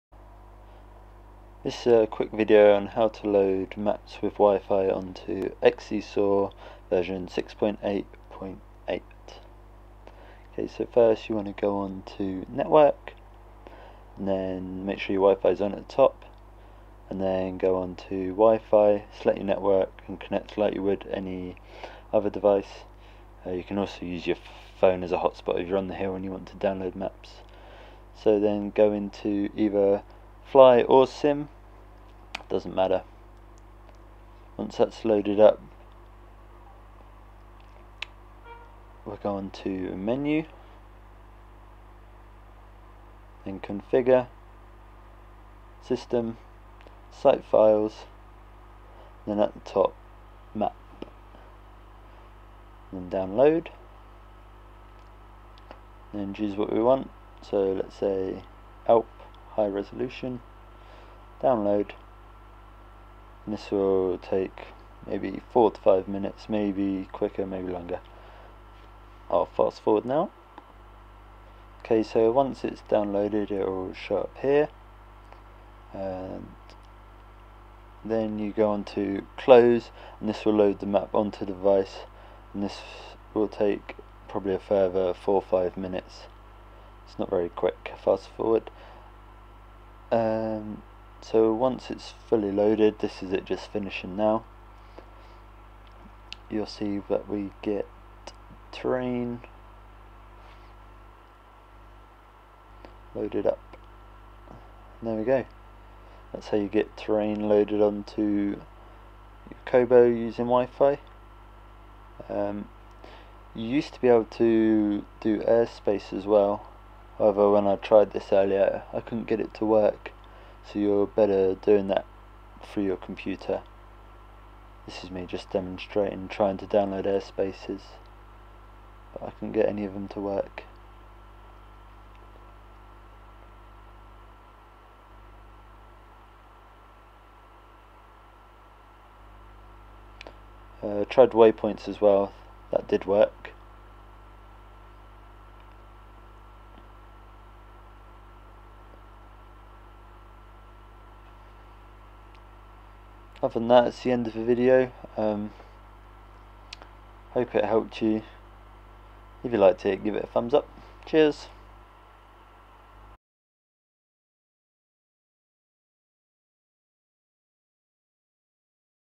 How to update Kobo XCSoar maps with Wifi
A short video on how to update maps using wifi on a Kobo running XCSoar